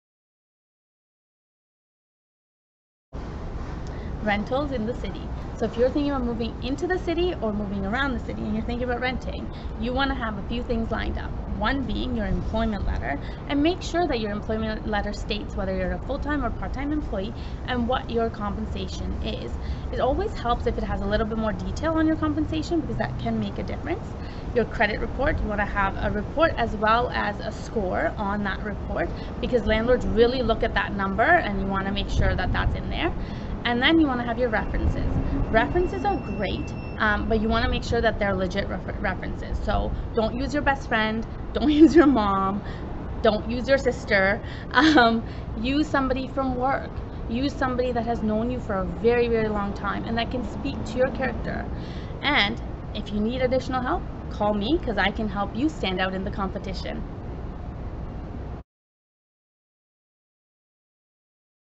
Moving to Toronto - Renting in the City
As you know I'm a Real Estate Broker in Toronto, but I'm also a landlord. I have worked with lots of people to help them move into the city, and I have found and secured great tenants for my own properties.

Watch this short video to understand what it takes to move into a quality rental property in Toronto.
- Nikku